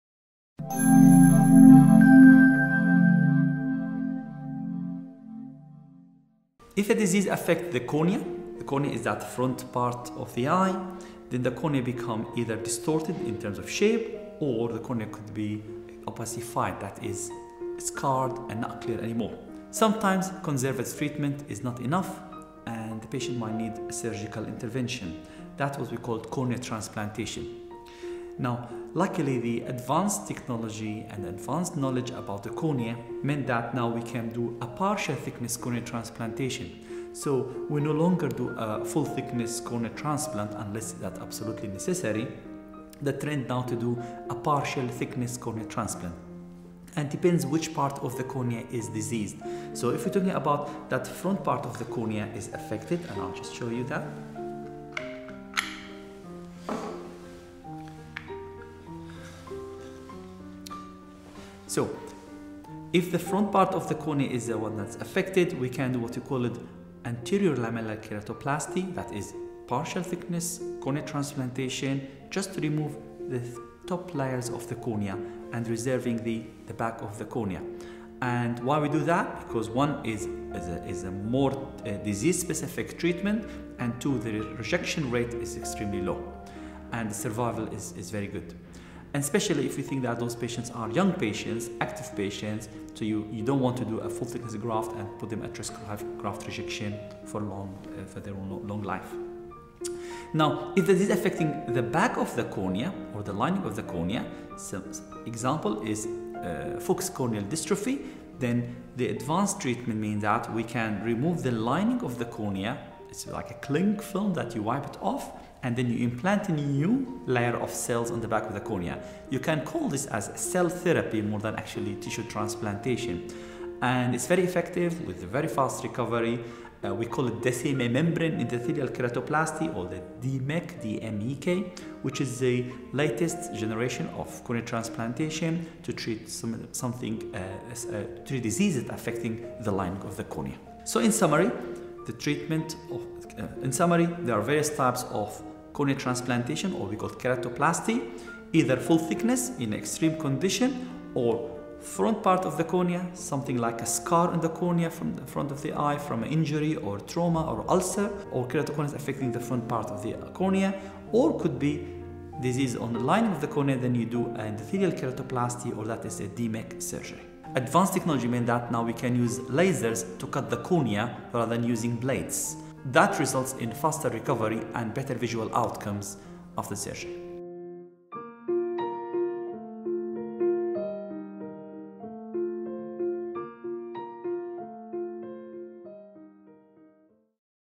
What is a corneal transplant and when is a corneal transplant needed?
A cornea transplant is an operation to remove all or part of a damaged cornea and replace it with healthy donor tissue. In this video, Mr Samer Hamada describes, in detail, what a corneal transplant is and when this treatment may be necessary.